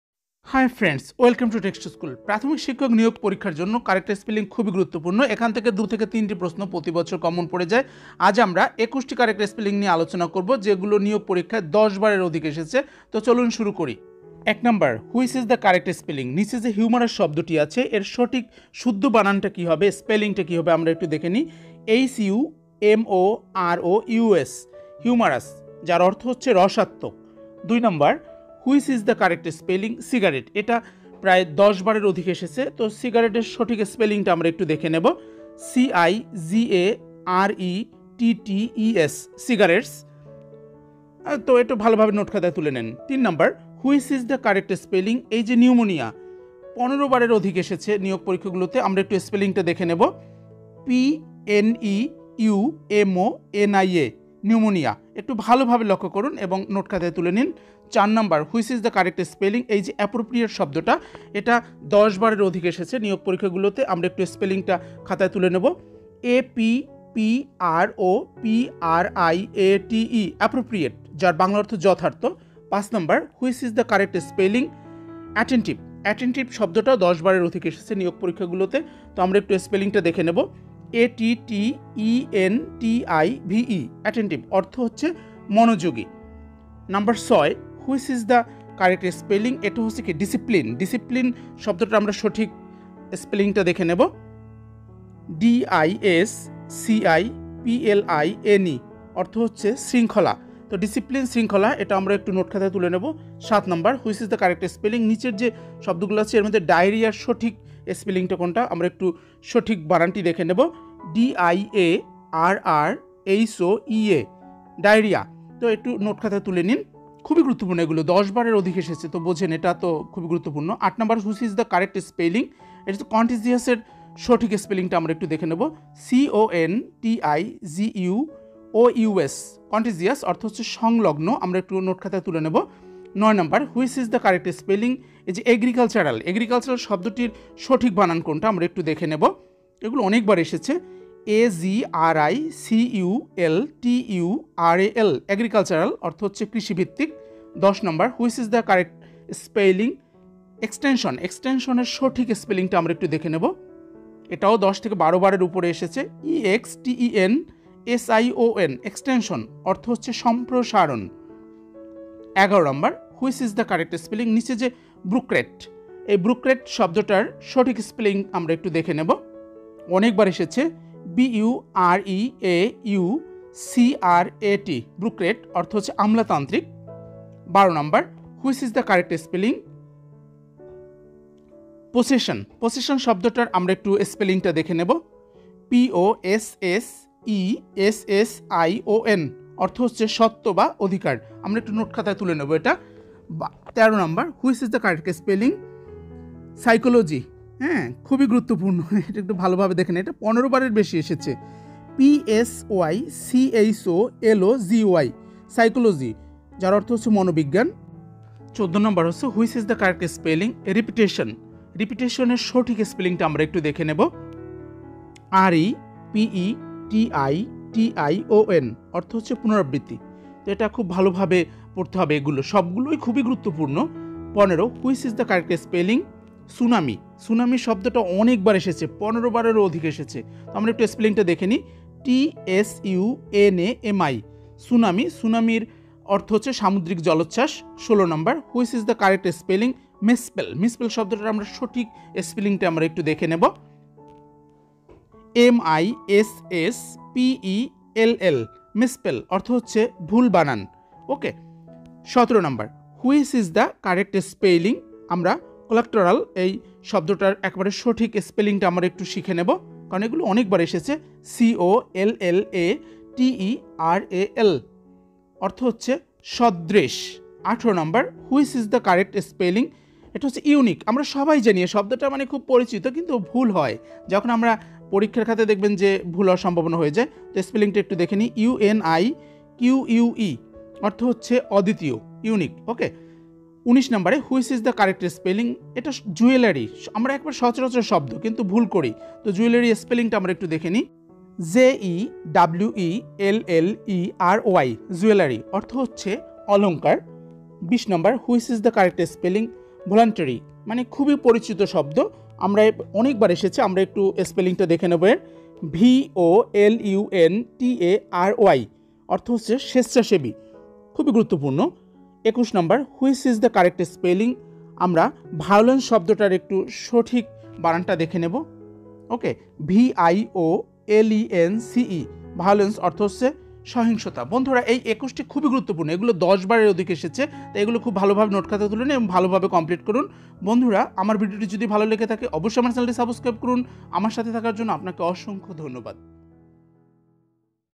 Most important 21 Correct Spelling for primary job candidates || প্রাইমারি নিয়োগ প্রস্তুতি ইংরেজি
This is a new video about Most important Correct Spelling for primary job candidates. প্রাইমারি শিক্ষক নিয়োগ প্রস্তুতি ইংরেজি 2021, প্রাথমিক শিক্ষক নিয়োগ প্রস্তুতি 2021 ইংরেজি |